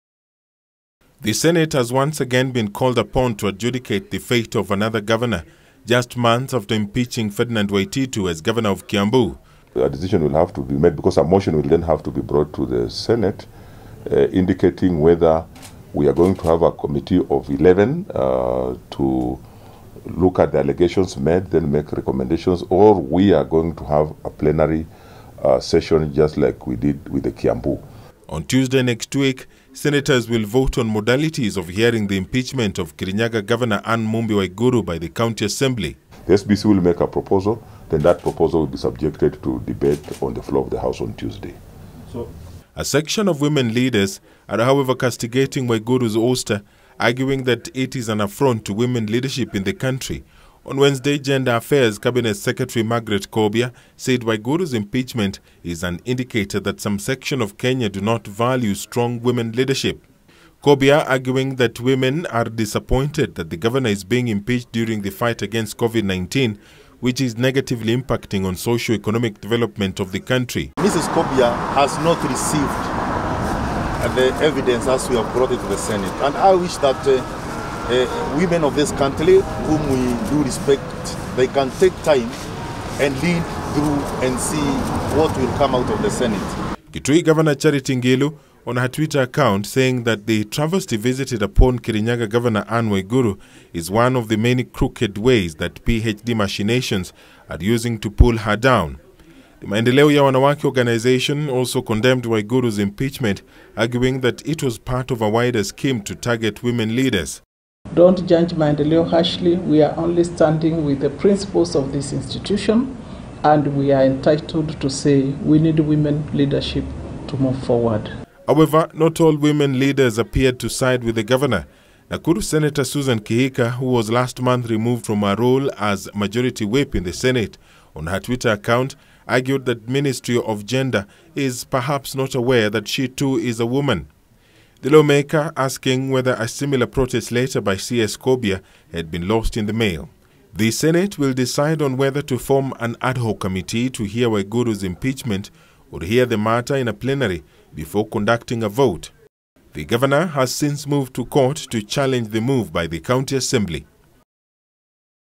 Women leaders support embattled Kirinyaga Governor Ann Waiguru following her impeachment
Various women leaders have come out to support embattled Kirinyaga Governor Ann Waiguru following her impeachment by the County Assembly. Led by Gender Affairs Cabinet Secretary Margaret Kobia, the leaders have argued that Waiguru’s ouster is an indicator that some Kenyans do not value strong women leadership. The fate of the governor now lies with the Senate after Speaker Kenneth Lusaka on Wednesday confirmed being in receipt of the resolution of the impeachment motion.